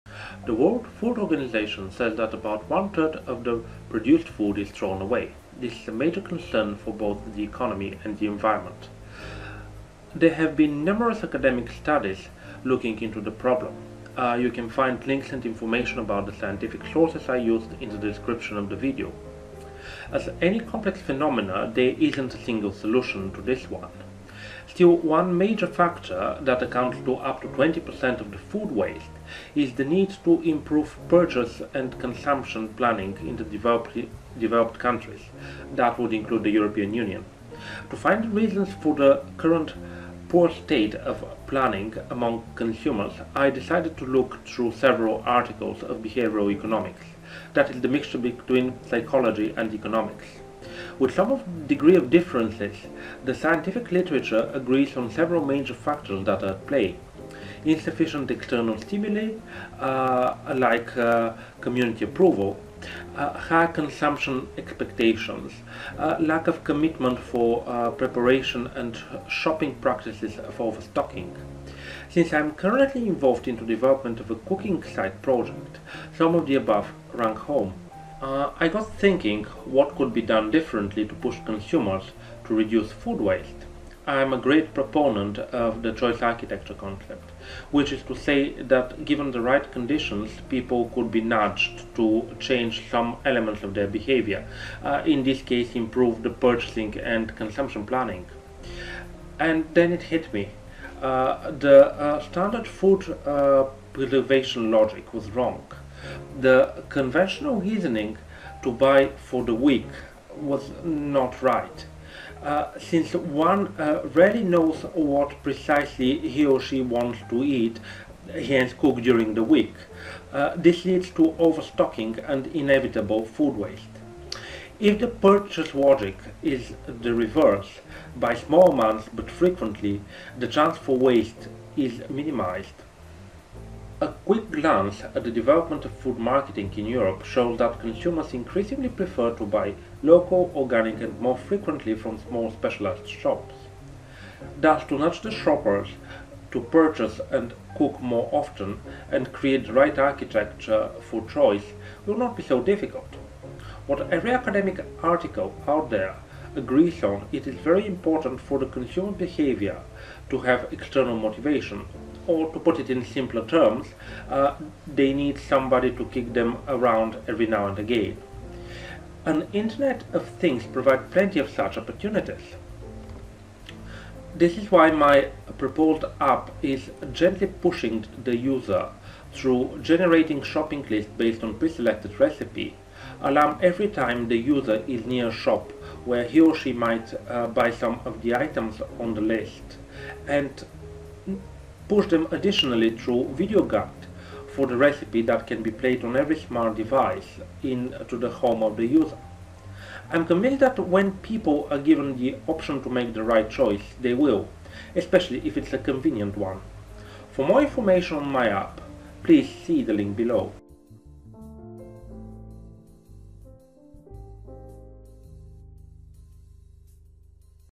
science behind the app
This video is part of Feel Good Food app

Links to the scientific sources used in the presentation:
3) Nelson, C et al. (2013) Climate change effects on agriculture: Economic responses to biophysical shocks ); Li, T. (2015) Uncertainties in predicting rice yield by current crop models under a wide range of climatic conditions
4) Among others Gustavsson, J. (2011) Global Food Losses and Waste
5) Quested, T.E. (2013)Spaghetti soup: The complex world of food waste behaviours ); Parizeau, K. et al. (2015) Household-level dynamics of food waste production and related beliefs, attitudes, and behaviours in Guelph, Ontario
6) Alliance expert report ; Giraud, G. (2014) Marketing Origin and Organic Labeled Food Products in Europe: Trade-Off Between Conviction and Convenience Stores
7) Vecchio, R. et al. (2016) Consumers' willingness to pay for conventional, organic and functional yogurt: evidence from experimental auctions ); Uvaneswaran, S. and B. Girimurugan (2016) International Trends and Buying Behaviour of Organic Food Products
8) Thaler, R. and C. Sunstain (2008) Nudge, Yale University Press
9) Hedhli, K et al. (2013) Shopping well-being at the mall: Construct, antecedents, and consequences ); Sunstain, C. (2015) Choosing not to choose, Oxford University Press